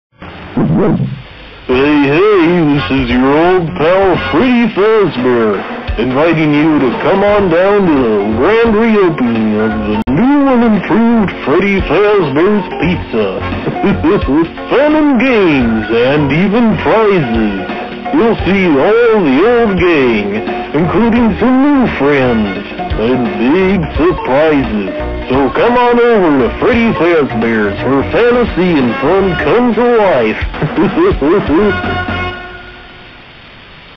Grand-Reopening Commercial [FNAF/VHS]
Hey guys I know you guys probably have a lot of questions regarding my disappearance with not only me but with my videos as well and well I just want to say that I will be back with maybe a new series idk yet if it's a series or just spin offs and yes this are battington's models of FNAF 2 there not mine I will be using these models for my FNAF 2 vhs tapes because hey there unique anyways this will be like a little teaser for what's to come and I am officially back :)

Audio by Tymine